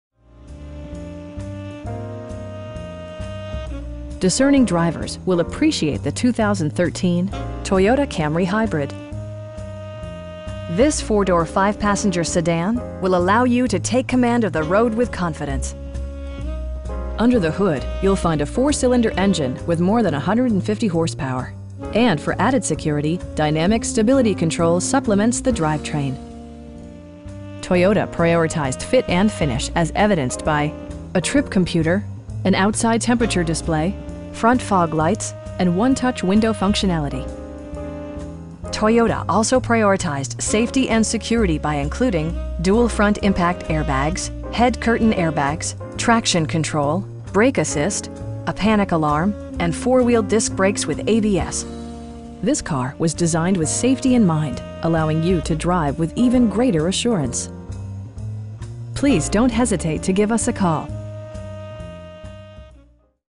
2013 Toyota Camry Hybrid XLE in Sanford, FL 32771
Discerning drivers will appreciate the 2013 Toyota Camry Hybrid. This 4 door, 5 passenger sedan will allow you to take command of the road with confidence! Under the hood you'll find a 4 cylinder engine with more than 150 horsepower, and for added security, dynamic Stability Control supplements the drivetrain. Toyota prioritized fit and finish as evidenced by: a trip computer, an outside temperature display, front fog lights, tilt steering wheel, power windows, cruise control, and 1-touch window functionality. Toyota also prioritized safety and security by including: dual front impact airbags, head curtain airbags, traction control, brake assist, a panic alarm, and 4 wheel disc brakes with ABS. This car was designed with safety in mind, allowing you to drive with even greater assurance. You will have a pleasant shopping experience that is fun, informative, and never high pressured. Please don't hesitate to give us a call.